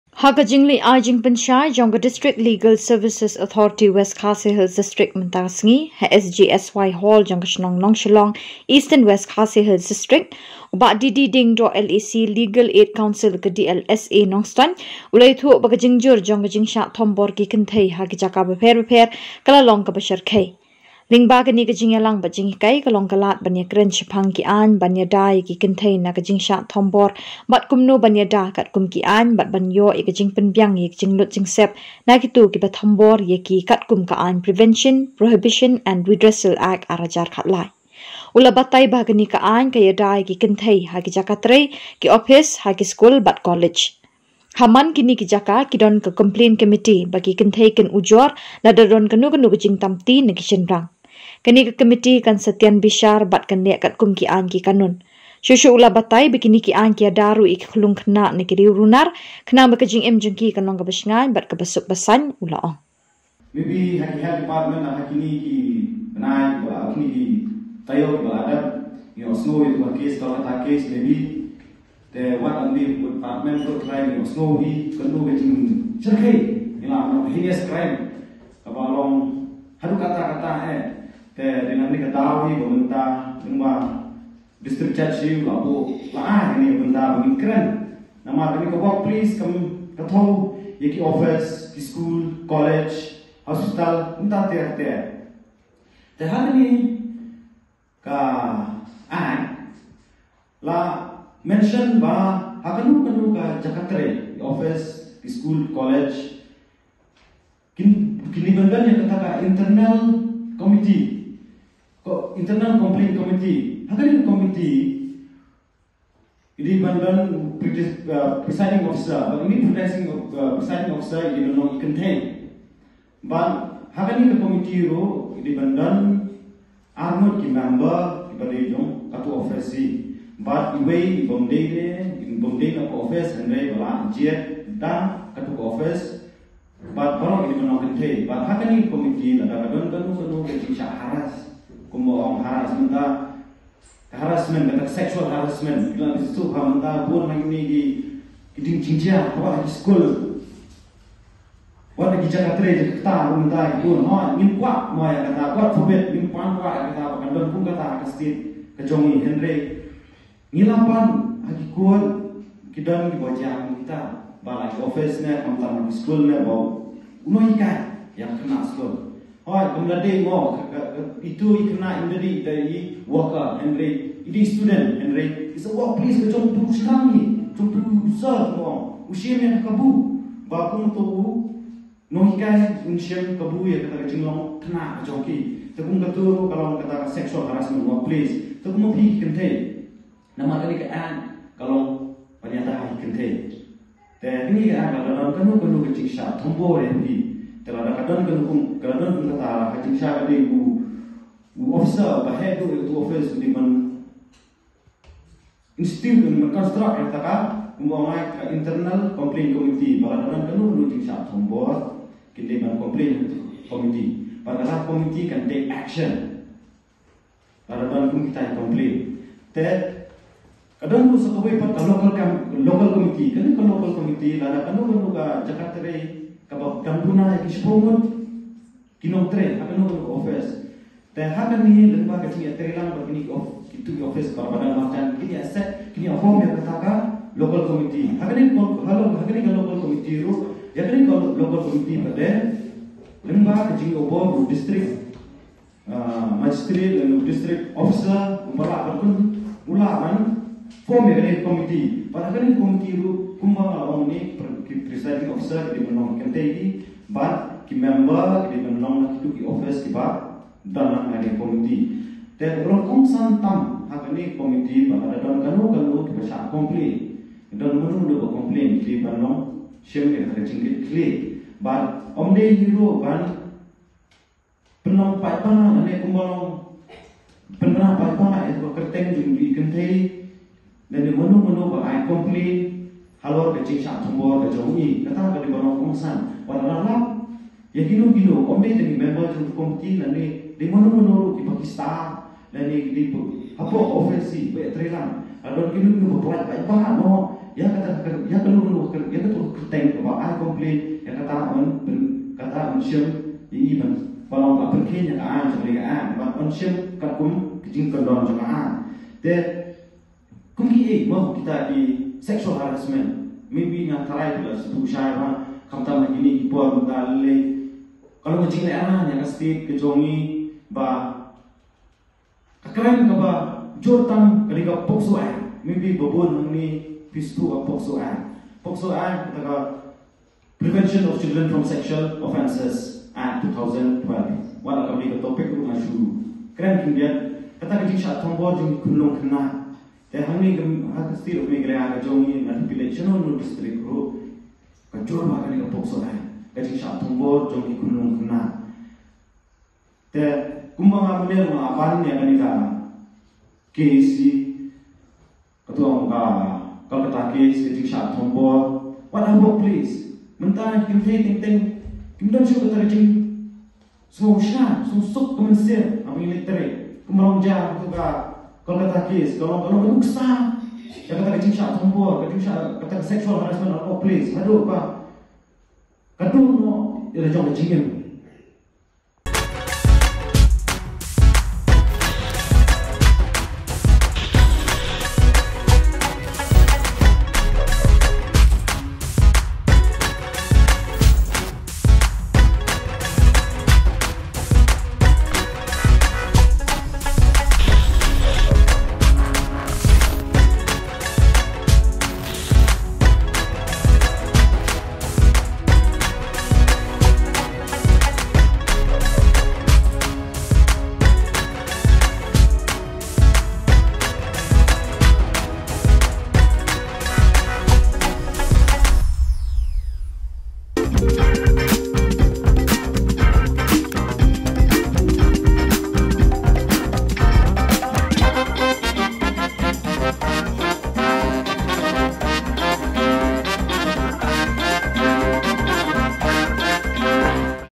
BUN KI KYNTHEI MYNTA KIM SHNGAIN SHUH :D D DIENGDOH
Ha ka jingleit ai jingpynshai jong ka District Legal Services Authority West khasi Hills District mynta ka sngi ha SGSY hall Jong ka Shnong Nongshillong, EWKHD, u Bah D.D Diengdoh (LAC) Legal Aid Counsel ka DLSA Nongstoiñ u la ïathuh ba ka jingjur jong ka jingshah thombor ki kynthei ha ki jaka bapher bapher kaba la long shyrkhei.  Lyngba kane ka jingialang  bad jinghikai ka long ka lad ban iakren shaphang ki ain ban iada ia ki kynthei na ka jingshah thombor bad kumno ban iada, katkum ki ain, ban ban ioh ia ka jingpynbiang ia ki jinglut jingsep na kito kiba thombor ia ki katkum ka ain Prevention, Prohibition, and Redressal Act, 2013.
U la batai ba ha kane ka ain ka iada ia ki kynthei ha ki jaka trei, ki Ophis, ha ki skul  bad college. Ha man kine ki jaka ki don ki don ka complaint Commitee ba ki kynthei kin ujor lada don kano kano ka jingtamti na ki shynrang. Kane ka Committee kan sa tian bishar bad kan leh katkum ki ain ki kanun.
Shuh shuh u la batai, kine ki ain ki iada ruh ia ki khyllung khynnah na ki riew runar khnang ba ka jingim jong ki kan long kaba sngain bad kaba suk basain, u la ong.
1. Local Cable LCN Channel 279
2. YouTube Channel
3. OTT @ dbmnTv